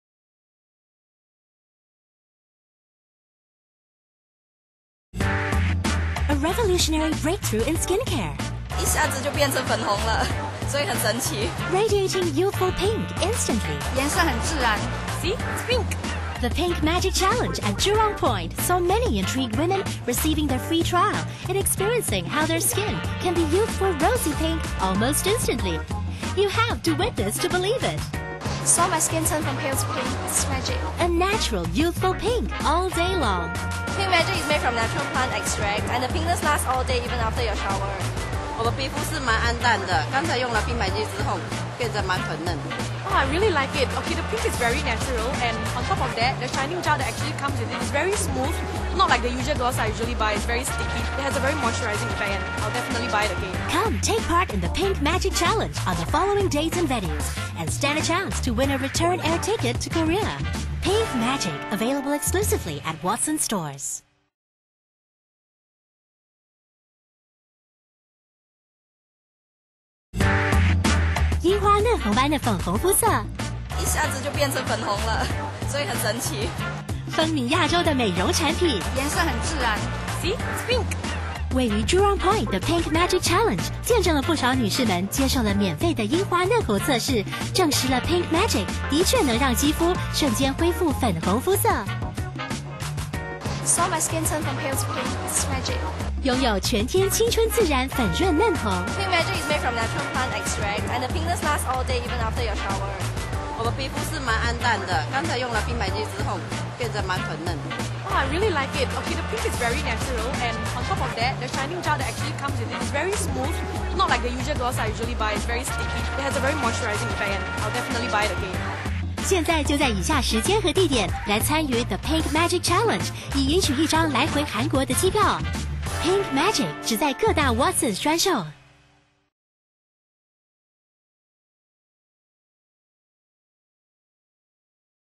Pink Magic
The latest revolutionary breakcare in Skincare! Pink Magic treats your skin to youthful rosy Pink in 1 Minute and the Pinkness lasts All Day Long! As seen so TV!